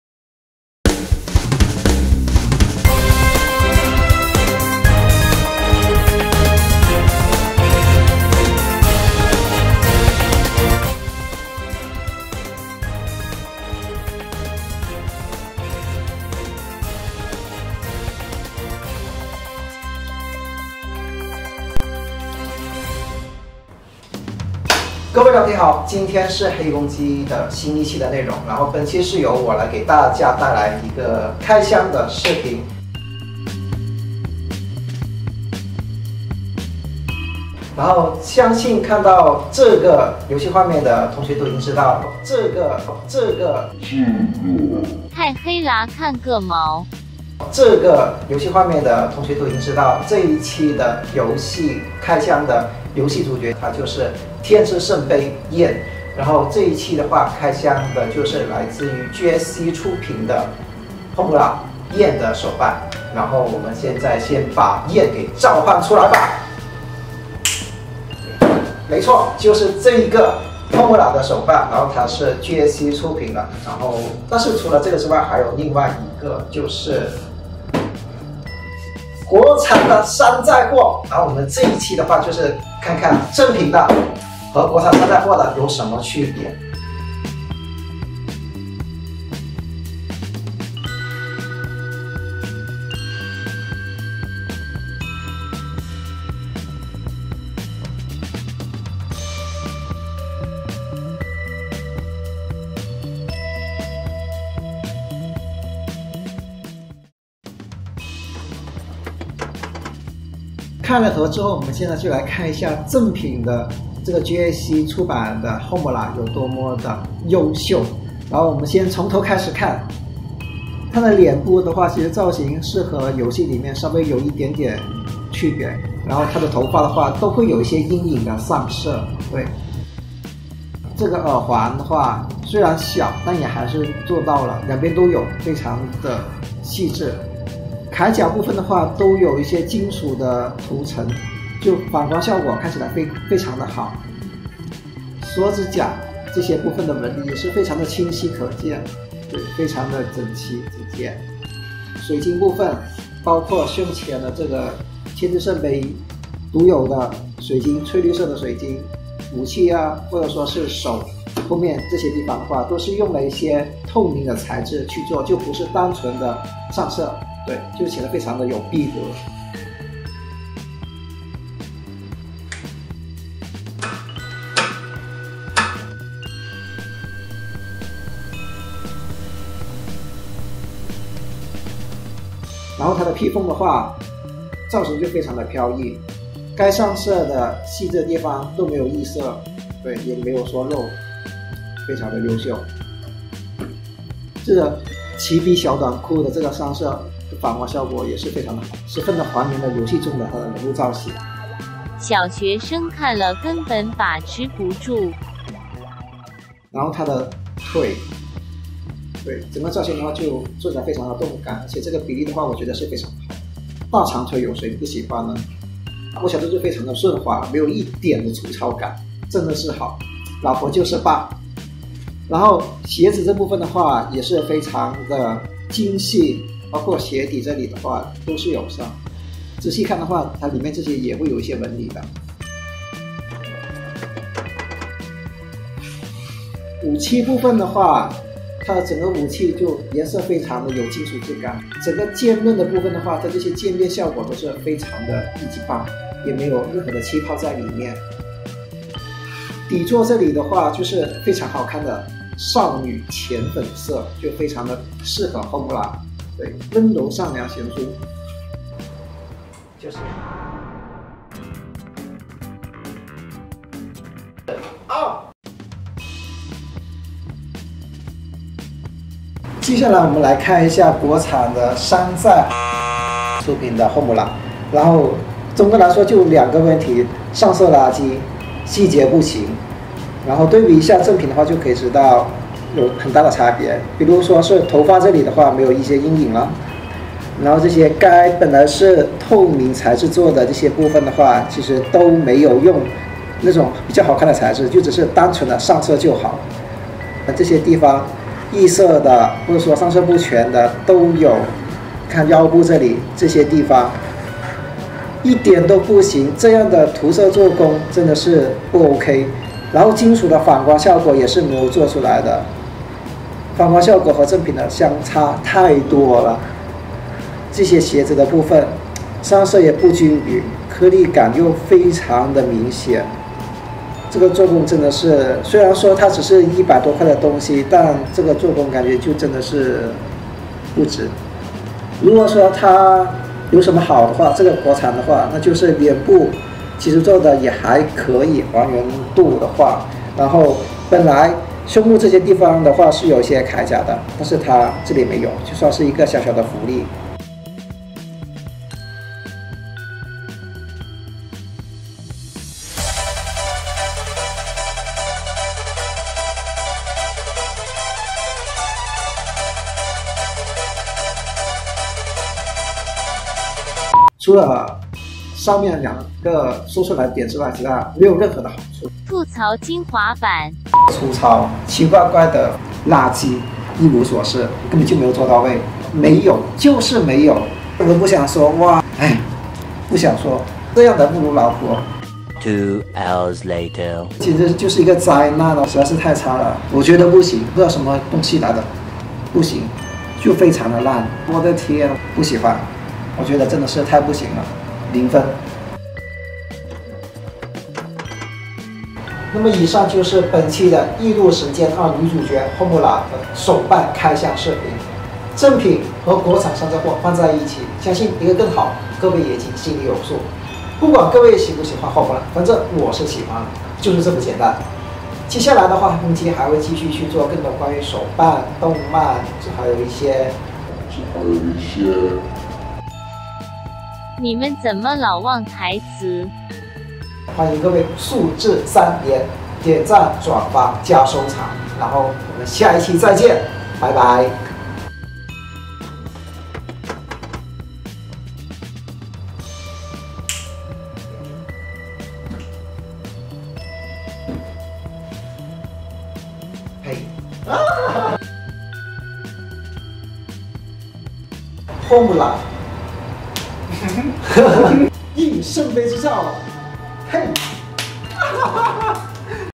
【BBC】异度之刃2焰开箱评测！100rmb的山寨老婆，真的OJBK吗？ | BigBoysClub
玩具界又一海景房，GSC异度之刃2焰，Big Boy Club首次开箱！
同时还有100rmb同款山寨版，横向对比
山寨老婆真的OJBK吗？
继续无限接近破产中，感谢支持(◒。◒)